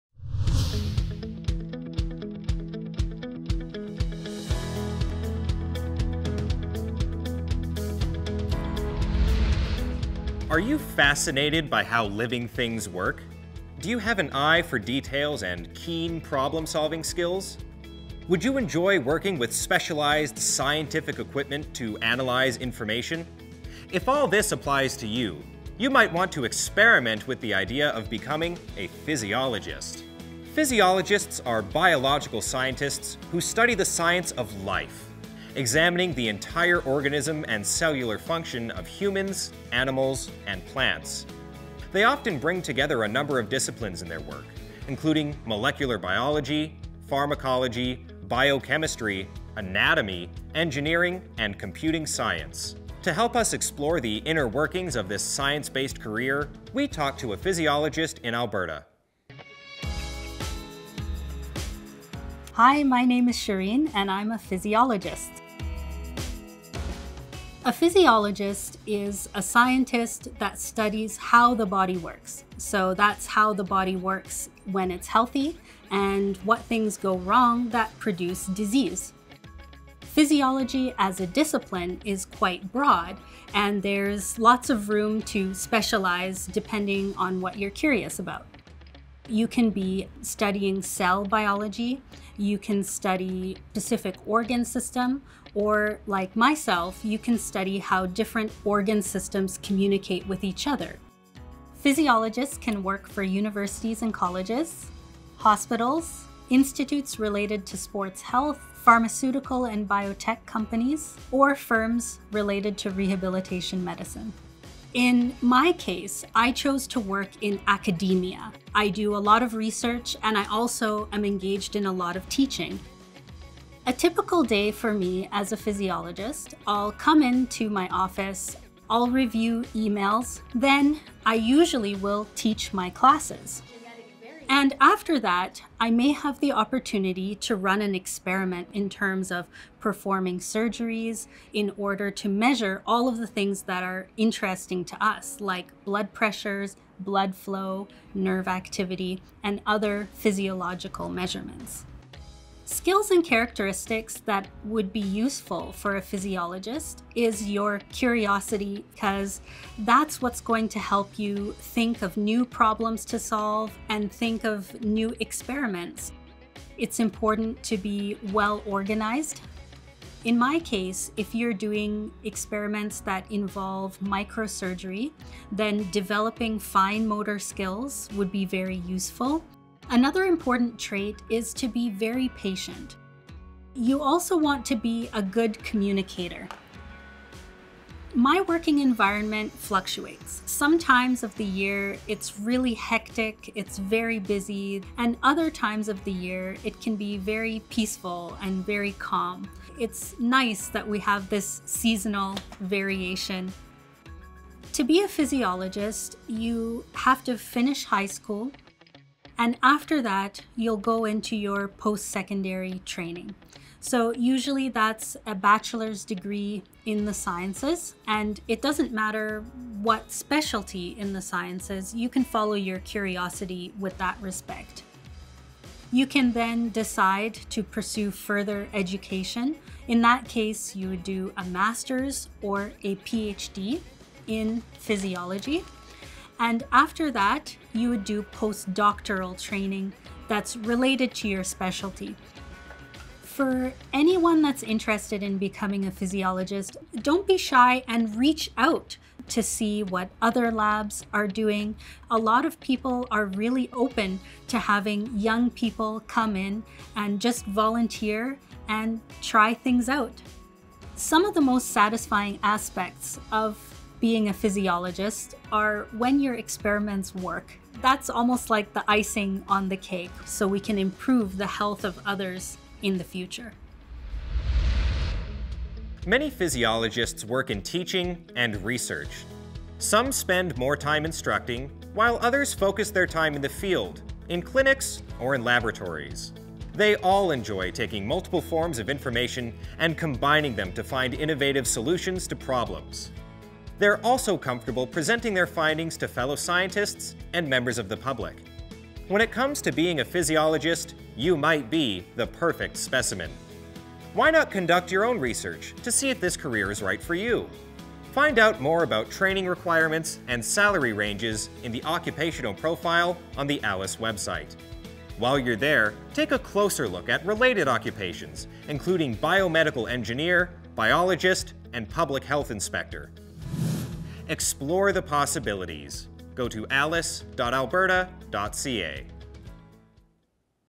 Occupational Video - Physiologist
Physiologists are scientists who study the whole organism and cellular function in humans, animals, and plants.

Find more details on what it's like to be a Physiologist